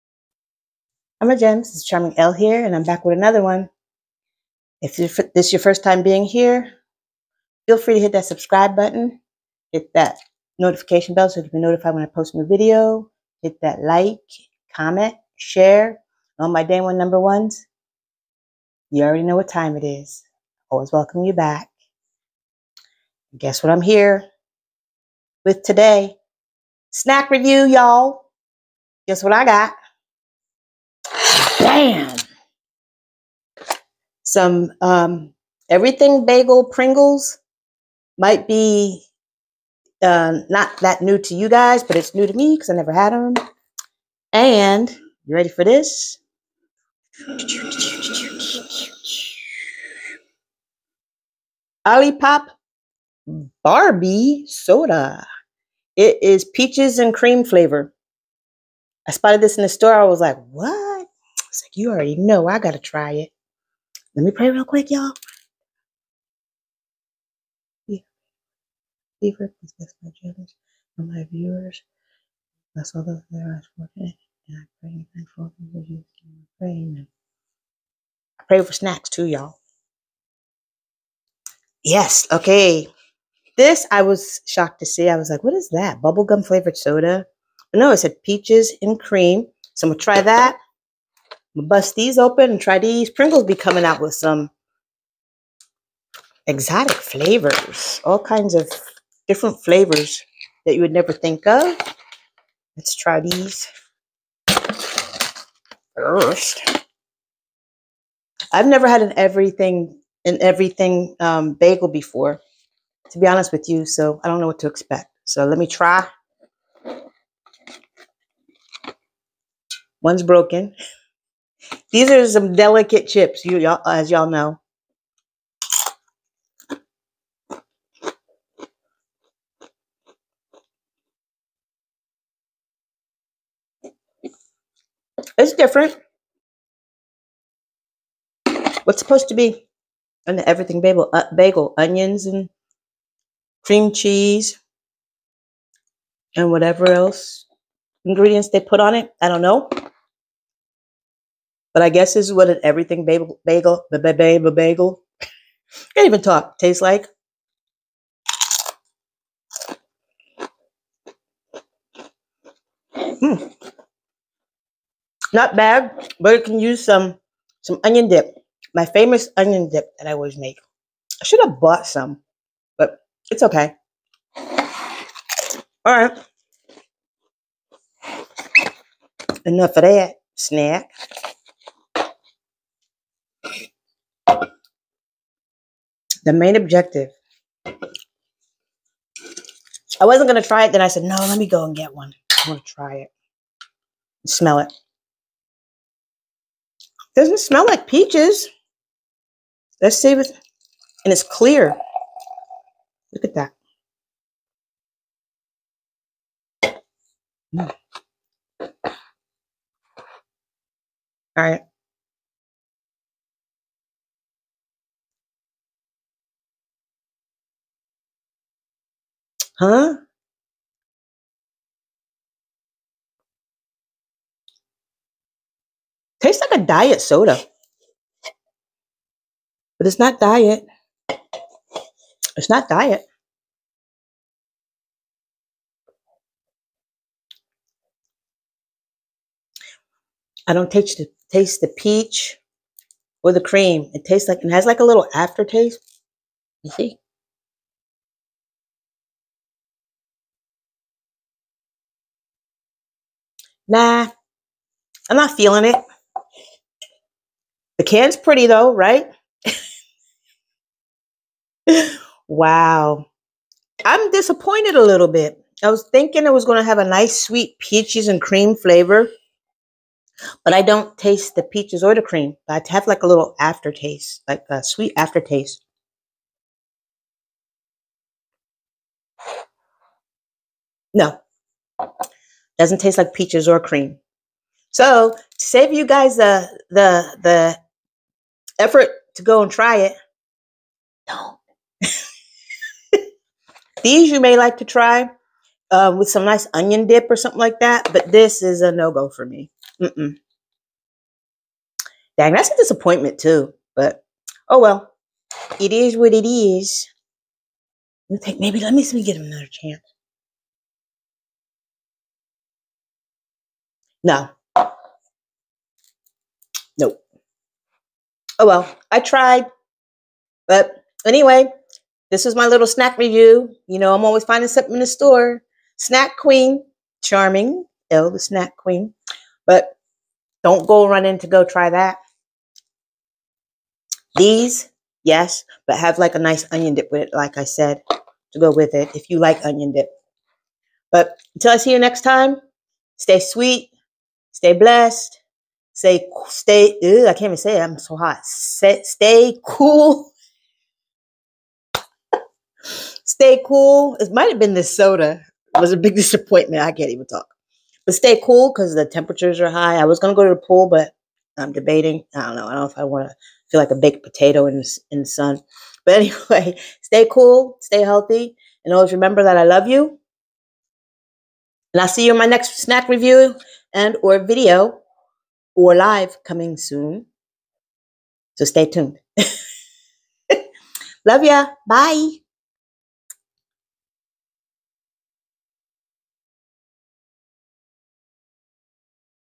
Snack time!! OLIPOP Barbie Soda ~ Pringles
Trying/reviewing the New OLIPOP Barbie Peaches n Cream Soda🥤 and Everything Bagel Pringles 🥯